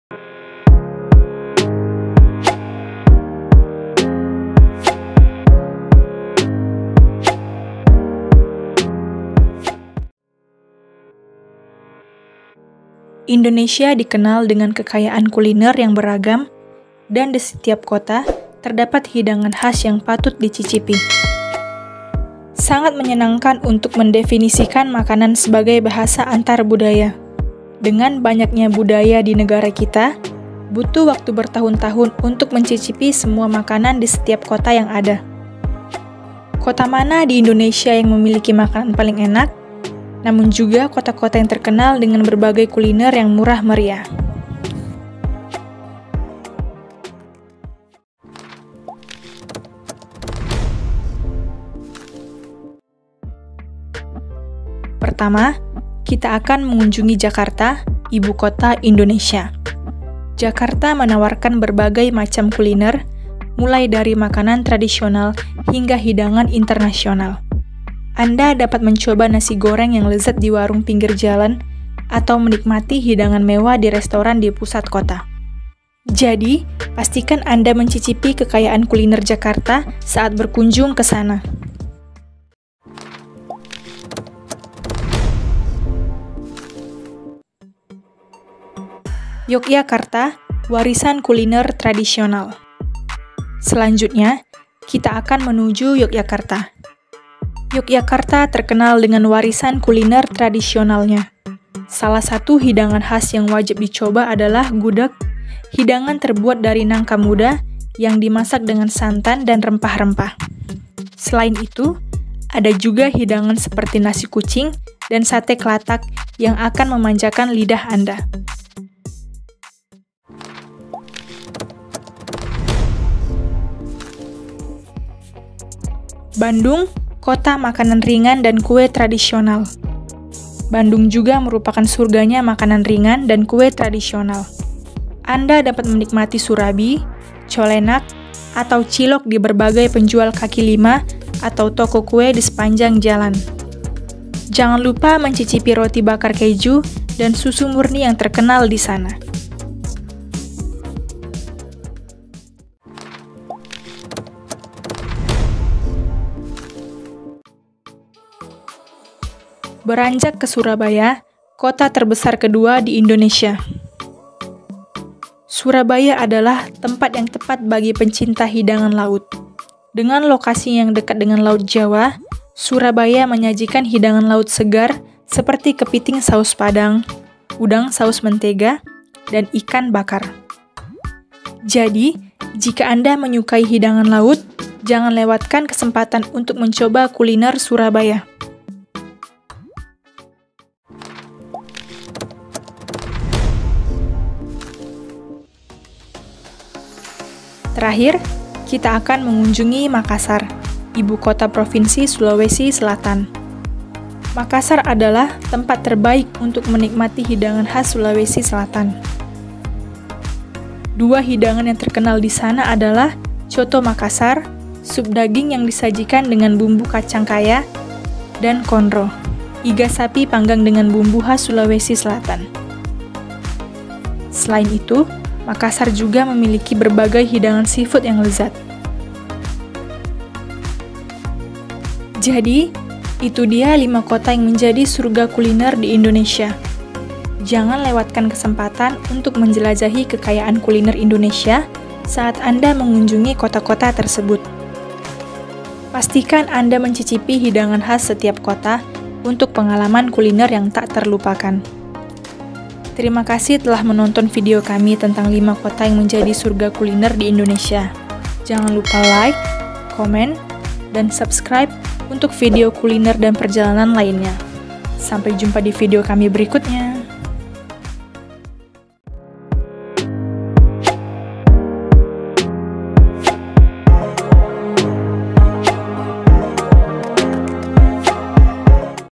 5 Kota yang Jadi Surga Kuliner di Indonesia !!
Rasanya memang tepat kalau kita menyebut makanan sebagai bahasa antar budaya. Dengan begitu banyaknya budaya di negeri kita ini, butuh bertahun-tahun untuk bisa menncicipi semua hidangan dari setiap kota yang ada. Kota mana saja sih yang menghidangkan makanan paling lezat di Indonesia? Bagi Loopers yang doyan makan dan merupakan petualang kuliner sejati, kamu harus pergi ke kota-kota ini sebelum ngaku sebagai seorang foodie.

 untuk info berita terkini dan paling menarik.

Jangan lupa juga SHARE video ini ke teman-teman Anda yang mungkin sedang membutuhkan informasi mengenai lainnya.